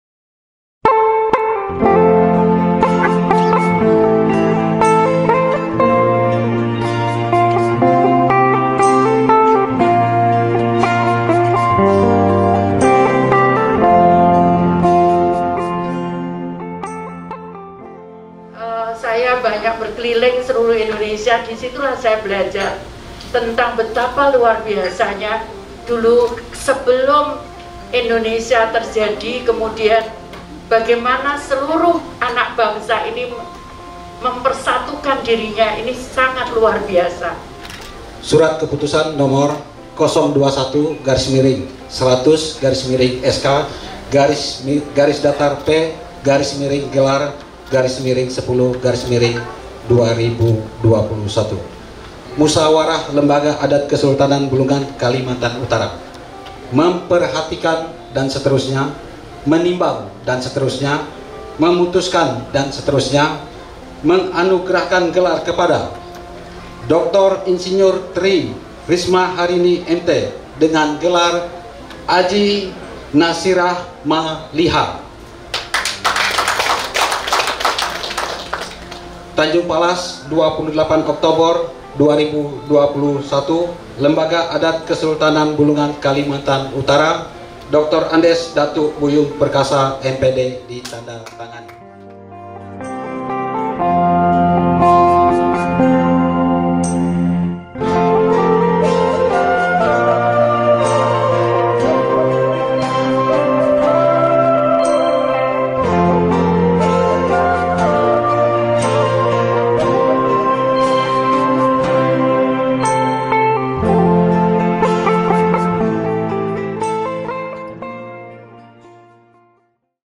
Dinilai sebagai Tokoh Pengayom, Kesultanan Bulungan Anugerahi Mensos Gelar Adat Adji Nasyrah Maliha
Kehadiran Menteri Sosial RI Tri Rismaharini membawa makna tersendiri bagi masyarakat, pemuda, dan para tokoh adat di Kesultanan Bulungan, Kalimantan Utara (28/10).

Mensos Risma dianugerahi gelar Adji Nasyrah Maliha yang disampaikan Ketua Pemangku Adat Bulungan, Datu Buyung Perkasa.

Gelar adat ini bermakna pemimpin yang berwibawa, pengayom, pelindung dan bijaksana dari Kesultanan Bulungan, Kalimantan Utara. Mensos mengaku terkejut dan terharu dengan gelar adat ini.